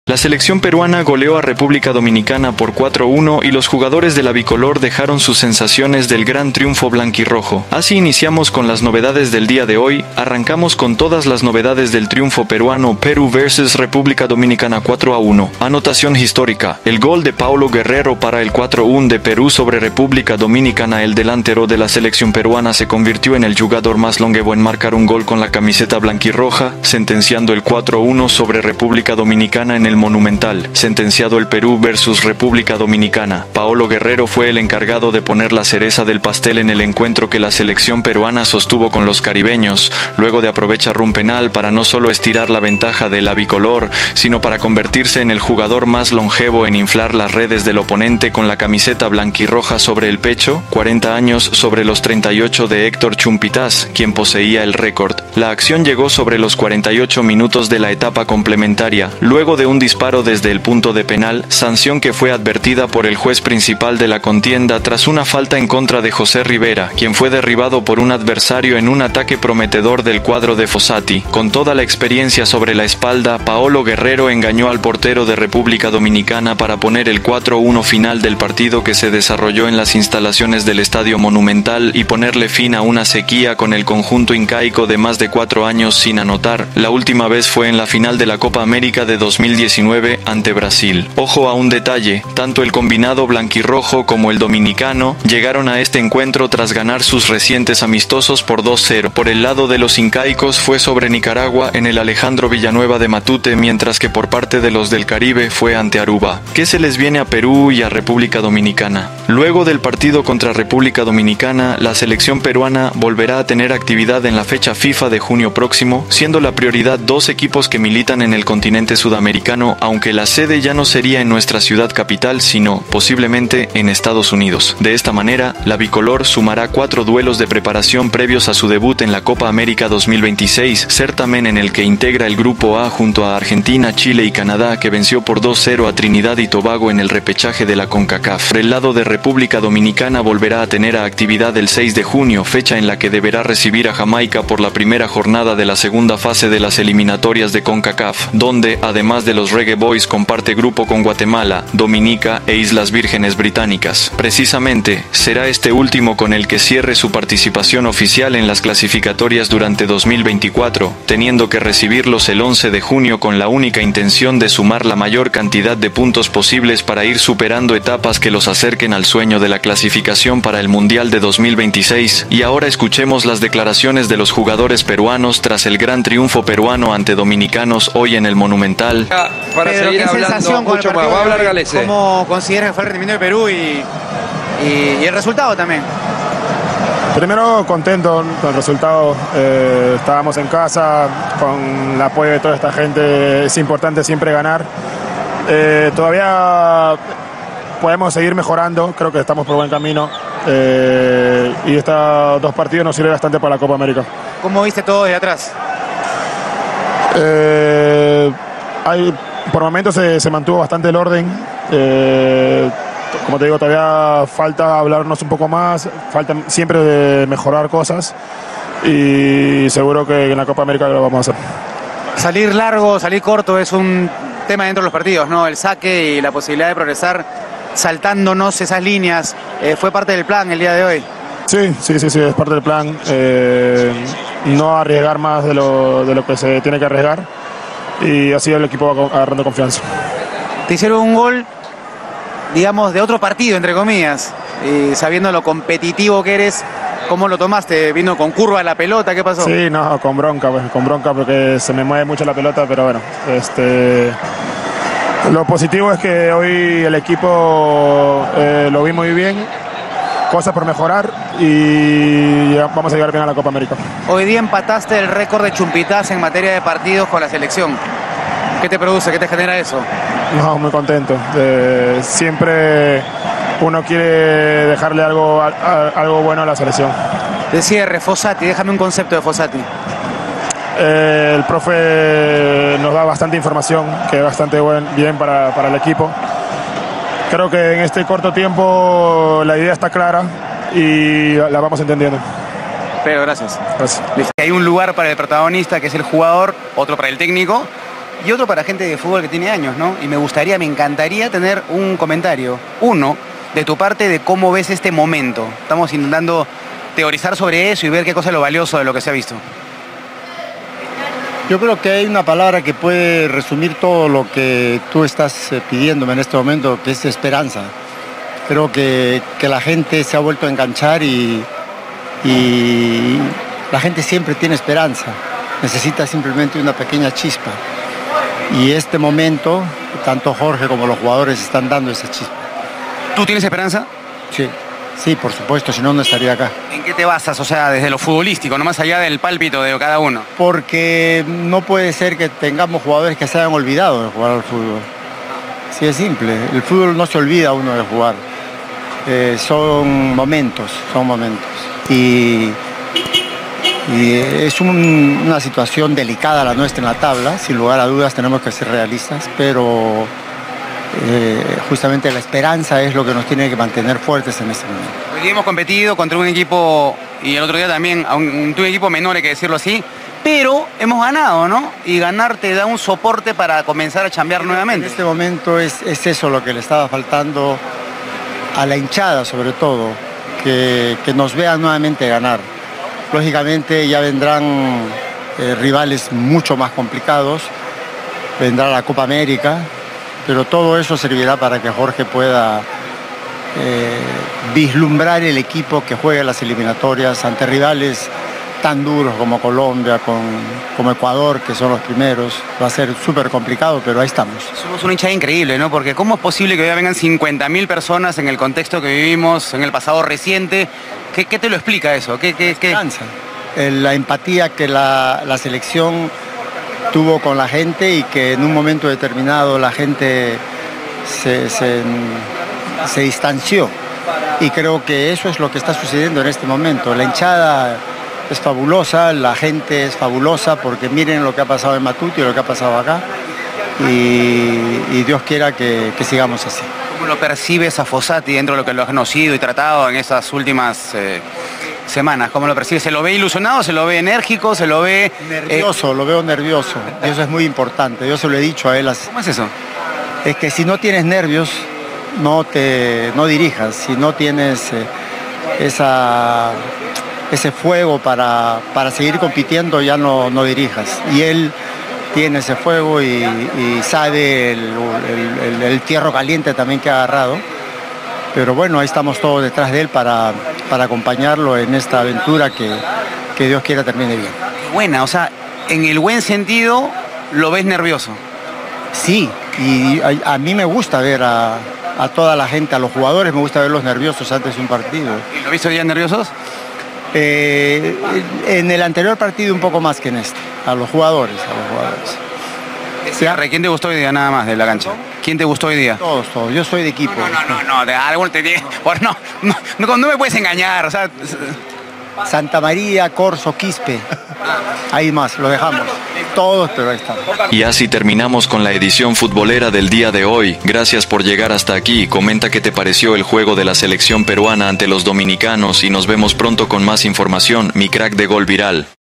GALLESE, PAOLO, OBLITAS Y JUGADORES PERUANOS EXT4SIAD0S tras GOLEADA! PERÚ 4-1 REP. DOMINICANA
EMOCIONANTES PALABRAS!!

JUAN CARLOS OBLITAS ENCANTADO CON NIVEL DE SELECCIÓN PERUANA Y GOLEADA DE PERÚ VS REPUBLICA DOMINICANA 4-1 en AMISTOSO!!

"CONFIEMOS EN FOSSATI ÉL NOS LLEVARÁ AL MUNDIAL 2026" ANALISIS EMOTIVO HOY OBLITAS!

PAOLO GUERRERO HABLA DE SU GOL, PEDRO GALLESE EMOTIVO ANALISIS HOY EN VIVO
GALLESE MOLESTO TRAS SU BLOOPER HOY EN VIVO

JUGADORES PERUANOS HOY ANALIZAN LA GOLEADA PERUANA HOY PERU VS REPUBLICA DOMINICANA

EMOCIONANTE ANALISIS HOY TREMENDA GOLEADA DE SELECCION PERUANA! PERU 4-1 DOMINICANA